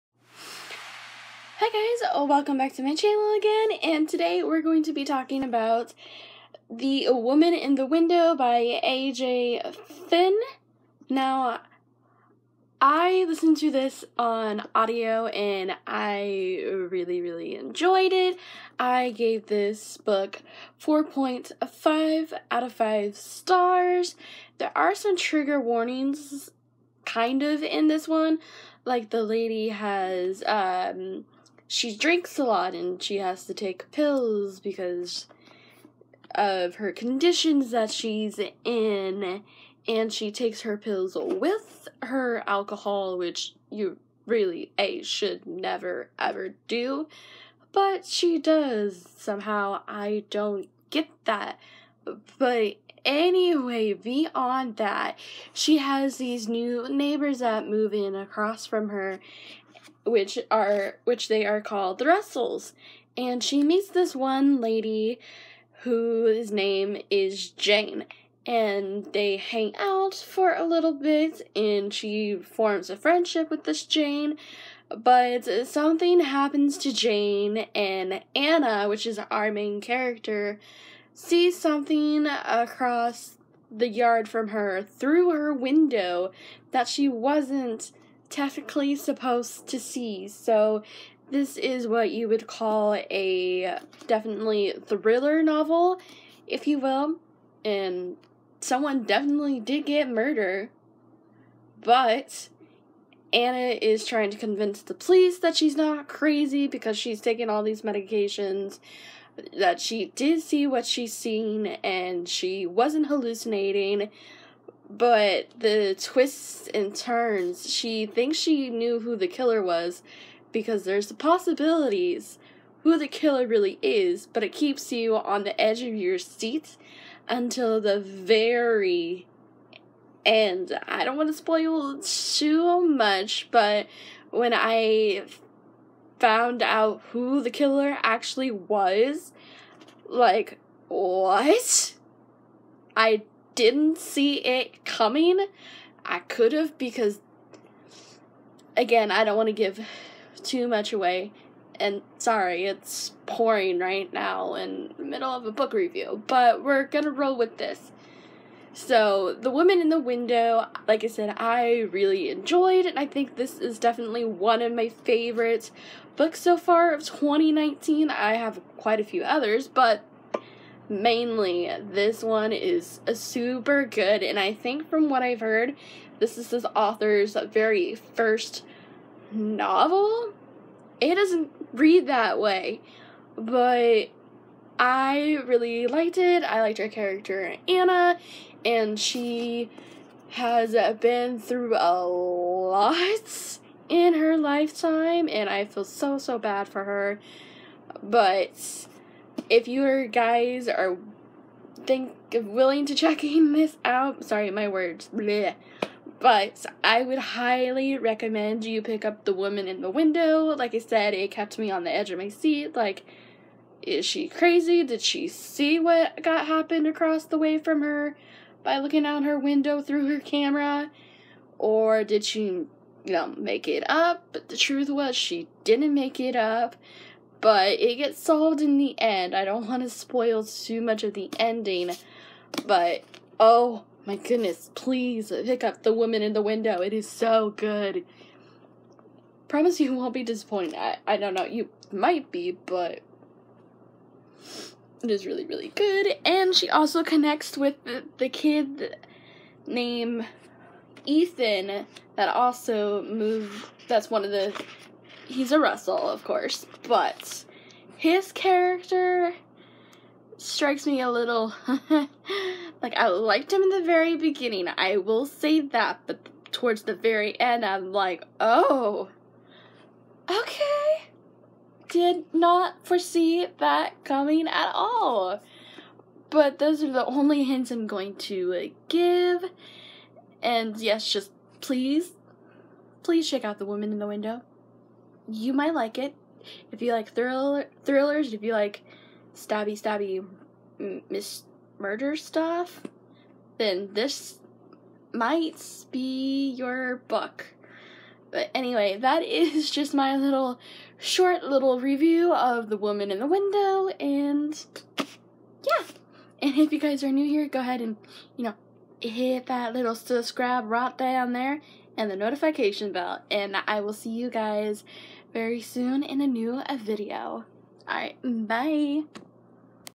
The woman in the window review
Here’s another short but quick review on how I liked this book 4.5 stars!! Please please check this book out I think you will like it.